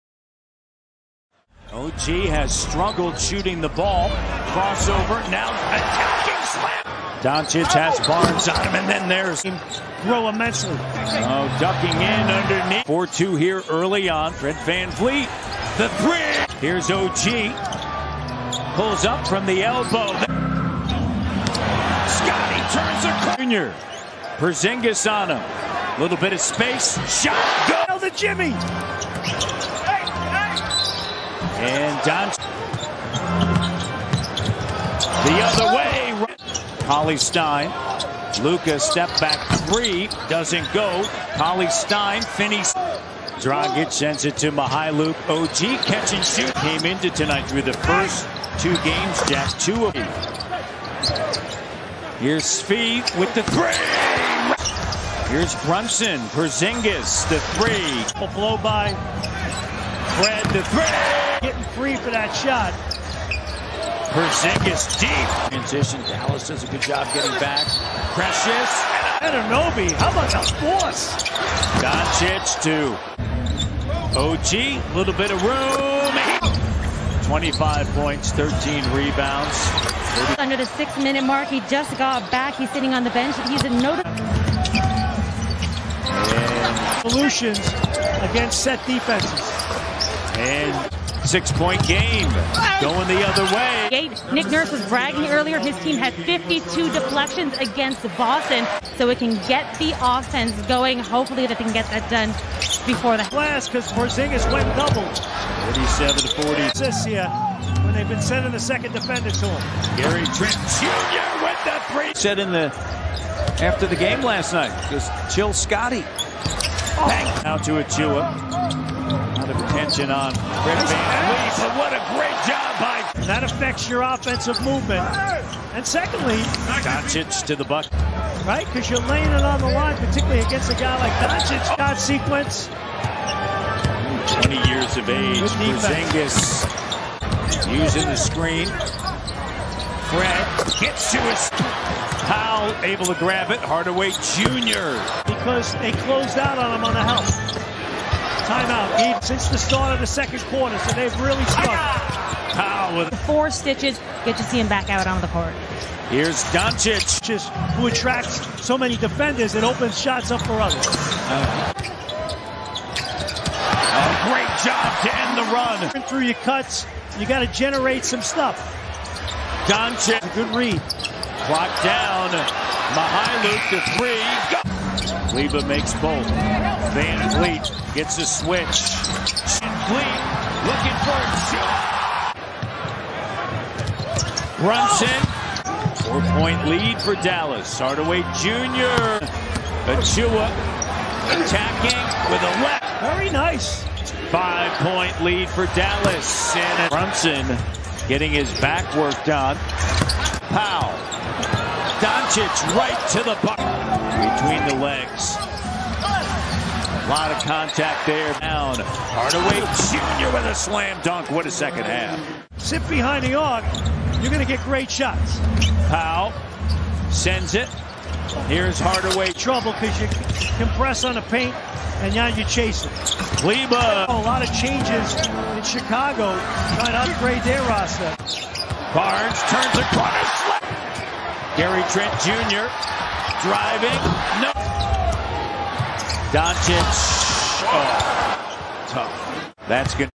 Mavericks at Raptors | FULL GAME HIGHLIGHTS |October 23, 2021
Dallas Mavericks at Toronto Raptors | FULL GAME HIGHLIGHTS | October 23, 2021 Dallas Mavericks were lead by Luka Doncic's 27PTS, 12AST, & 9REB.
Toronto Raptors Scottie Barnes had 17PTS & 8REB in the loss.

NFL,NBA,MLB,NCAA,NHL,XFL,HBO,PGA, & it’s broadcasters. I take the clips and compile them into popular plays.